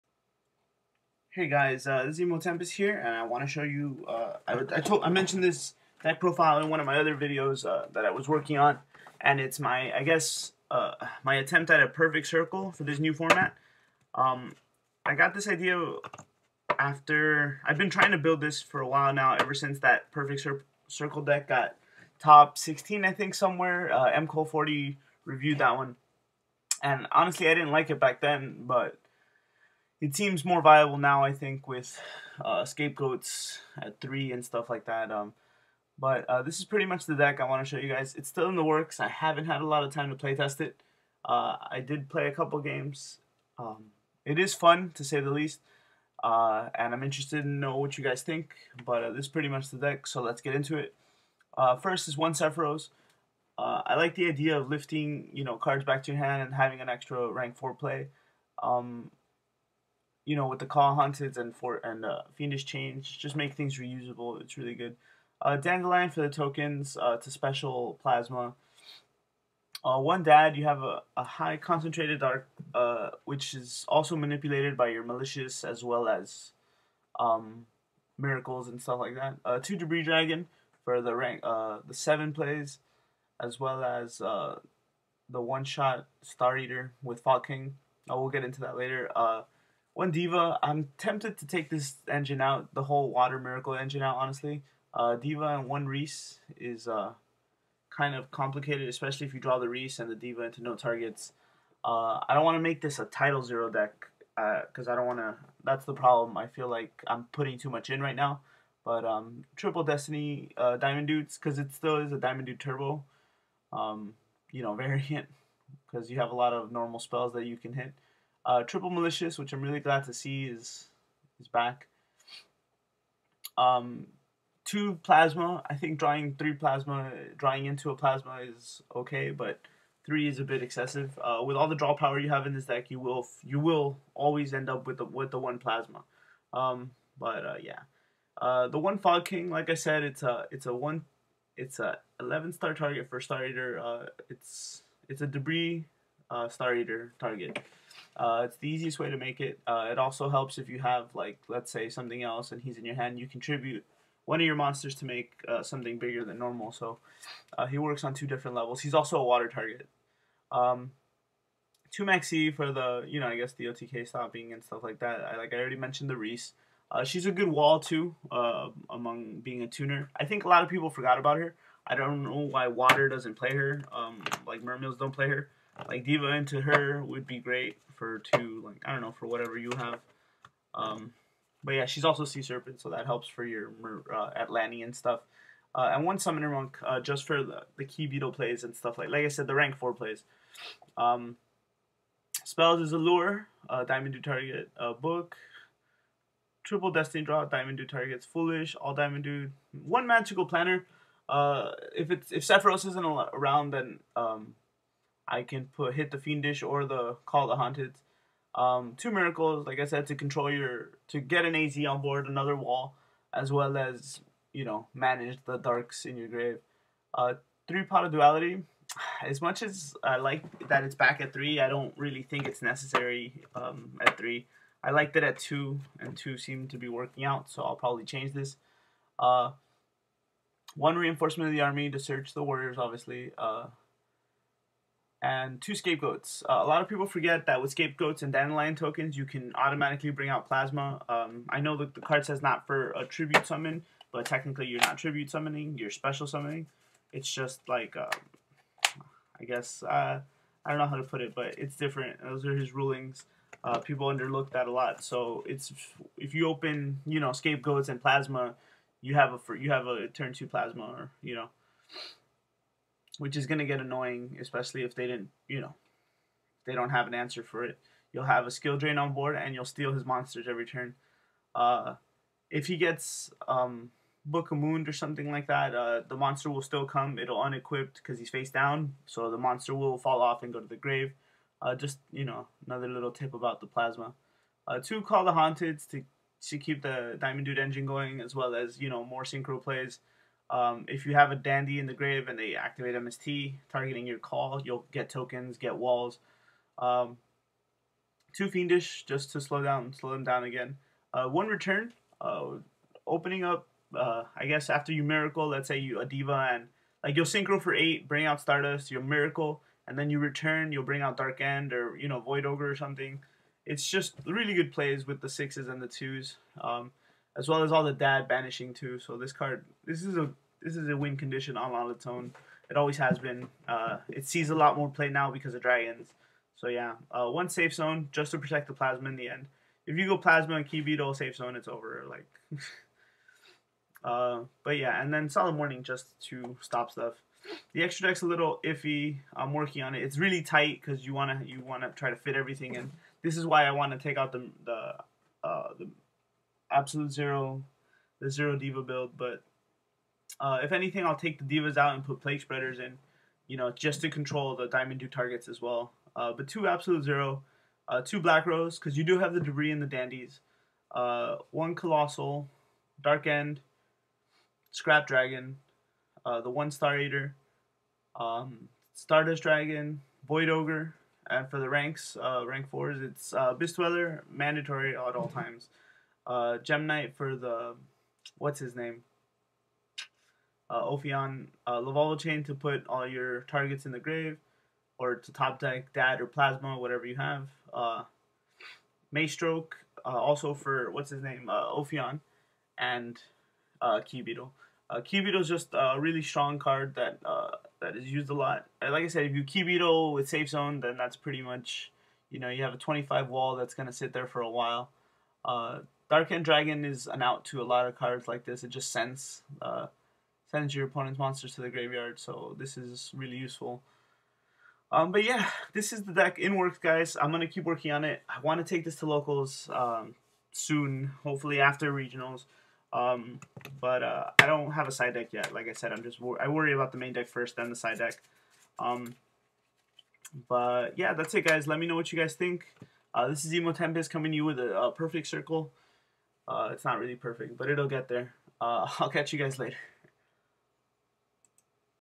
A Not So Perfect Circle...
Here is a destiny hero deck that I wanna work with as well and get ready for this meta. I feel like it is missing a few things but over all very viable this format. Open to any suggestions